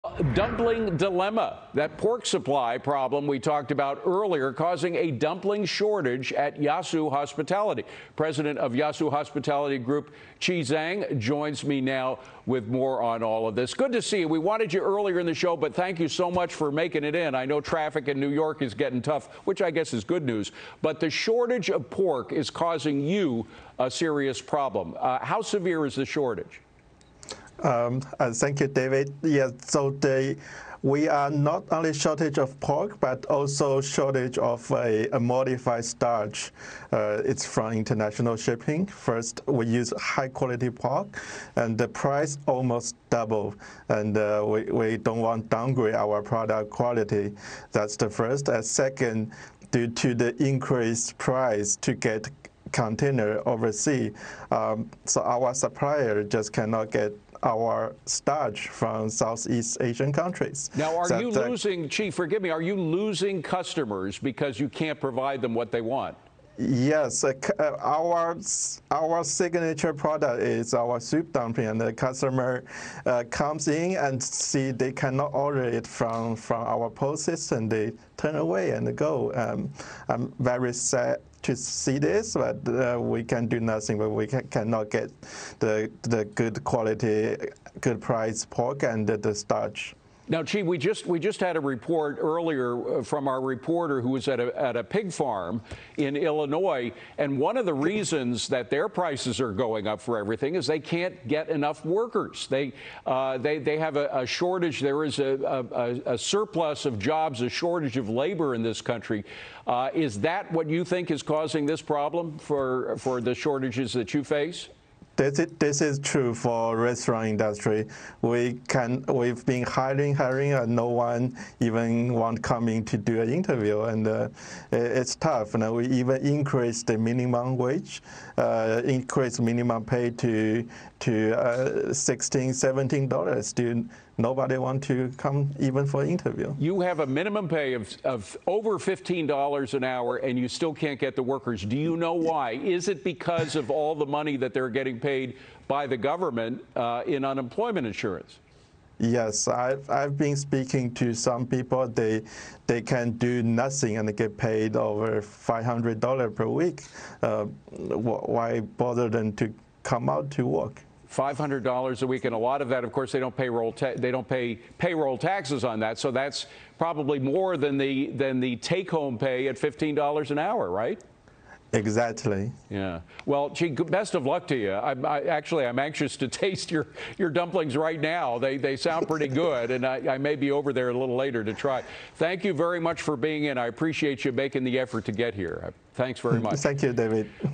Dumpling dilemma: Shortage caused by limited pork supply, rising prices
Yaso Hospitality Group President Chi Zhang discusses the dumpling shortage caused by limited pork supply and rising meat prices.